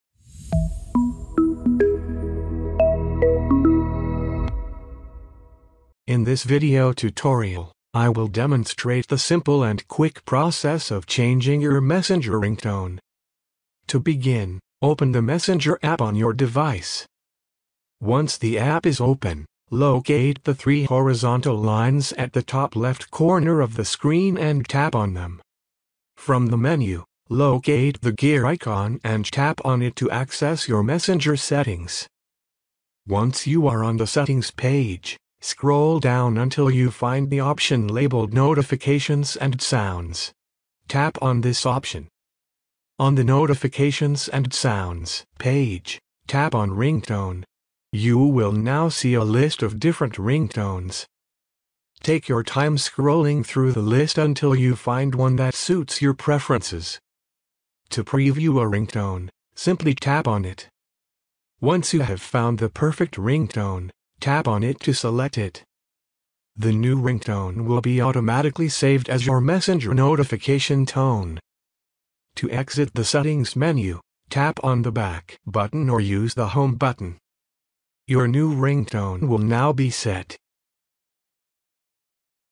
How To Change Messenger Ringtone
In this tutotrial i will show you how you can change messenger ringtone.